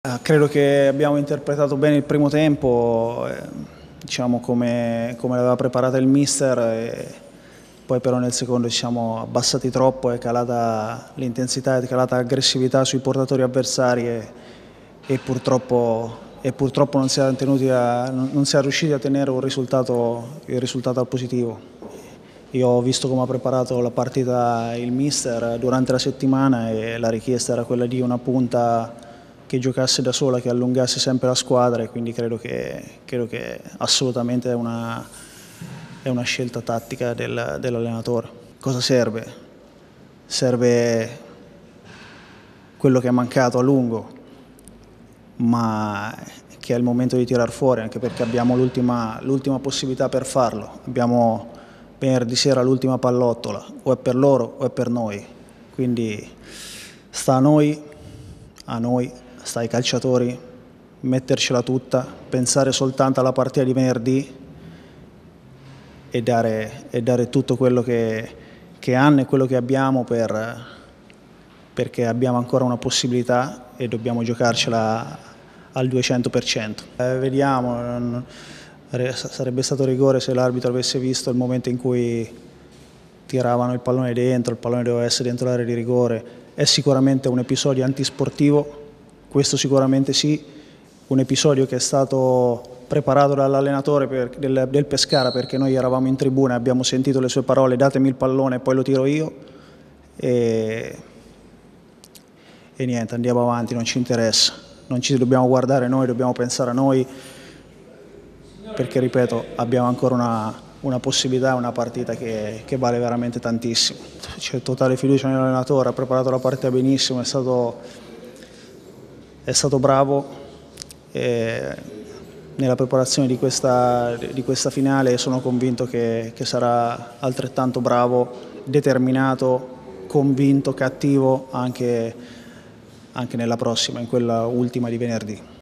Pescara - Perugia 2-1: Roberto Goretti
Playout - Serie B (gara d'andata). Conferenza post partita del direttore sportivo del Perugia